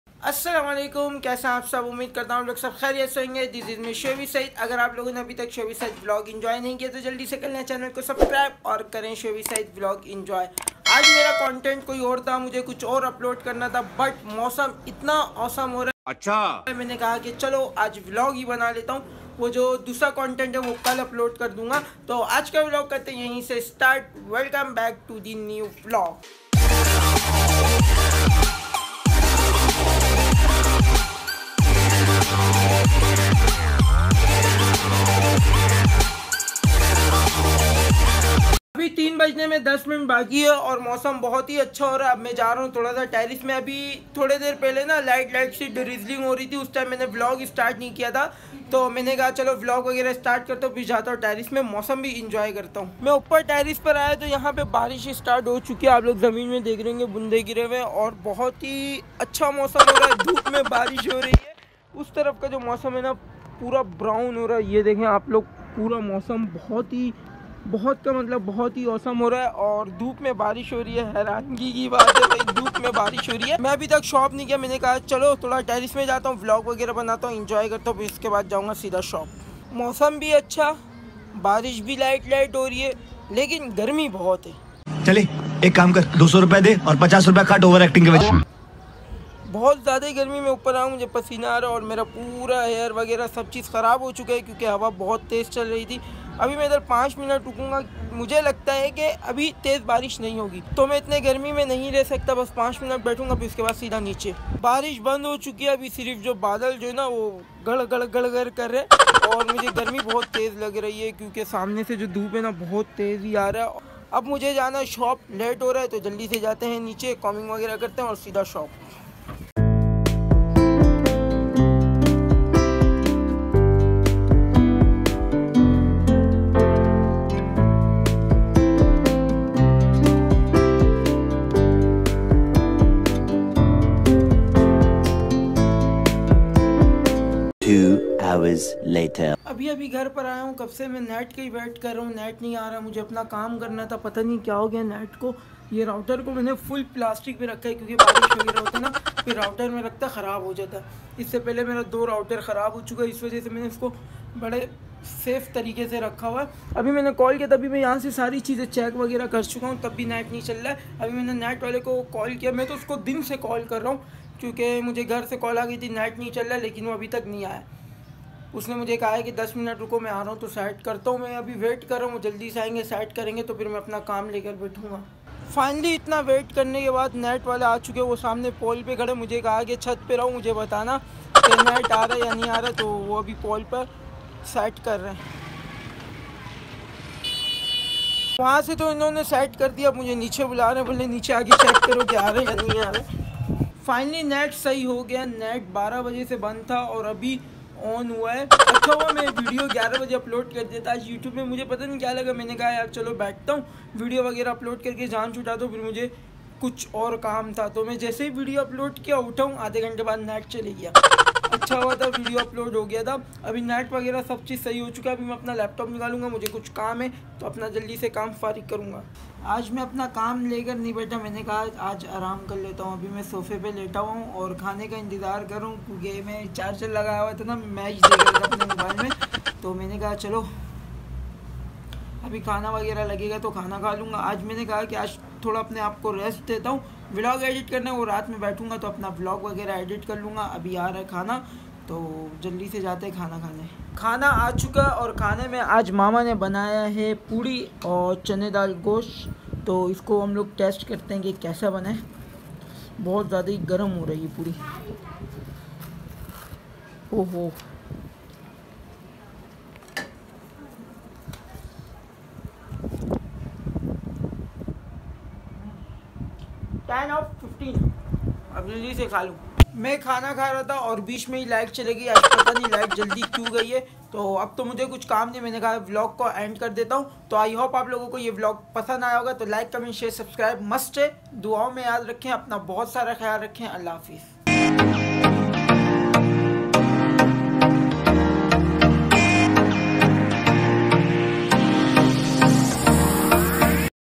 Karachi ke mosam ko achank se kia ho gaya😳
Shebby saeed Achank se karachi k mosam kharab hogaya